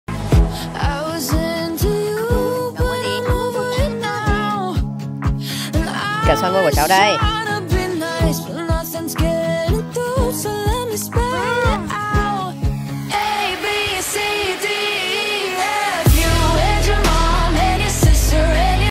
beggars and strawberry lipstick candy, watermelon ice cream, dinosaur eggs | Hmong family
shortsthey content in the video tells the story of poor, homeless people in poor rural areas
Do not copy videos or images in any form